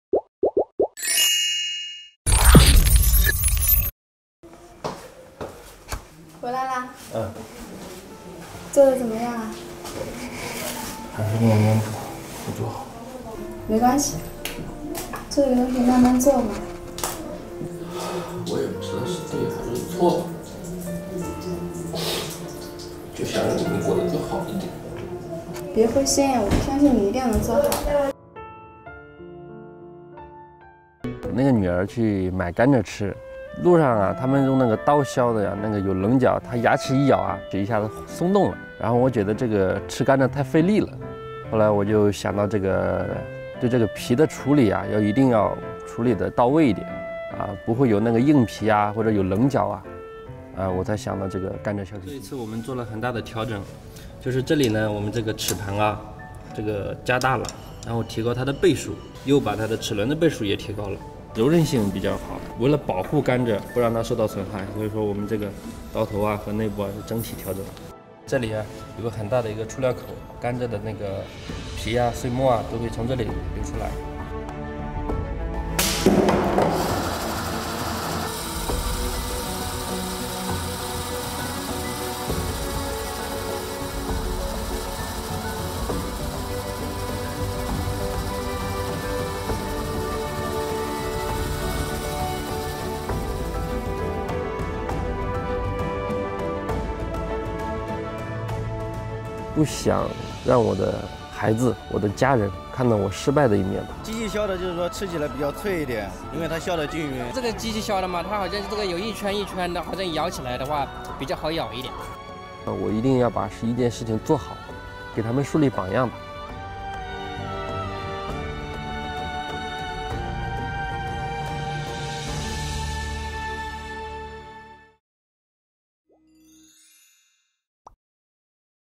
女兒吃甘蔗被劃傷，小伙一怒自製甘蔗削皮機，10秒削1根，無硬皮【發明迷】
小伙自製甘蔗削皮機